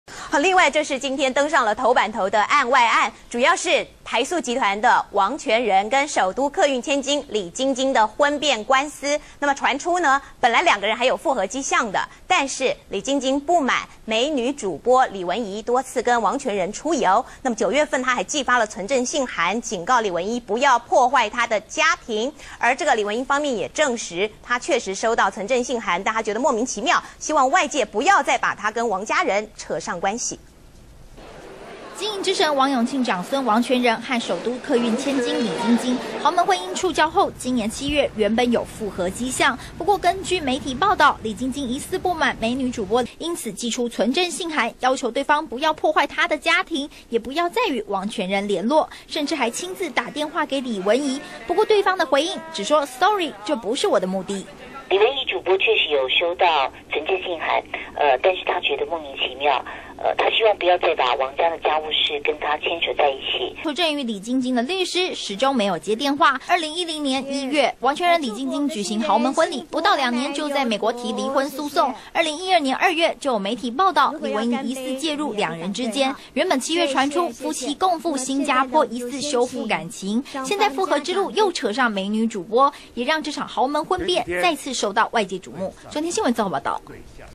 捍衛婚姻！傳李晶晶警告李文儀「別破壞我家庭」
中天新聞》王永慶長孫王泉仁，與首都客運千金李晶晶的婚變官司又有案外案，原本傳出有復合跡象，但有媒體報導李晶晶不滿美女主播李文儀多次與王泉仁出遊，9月寄發存證信函警告李文儀「別破壞我的家庭」，也掌握事證要向她提告求償。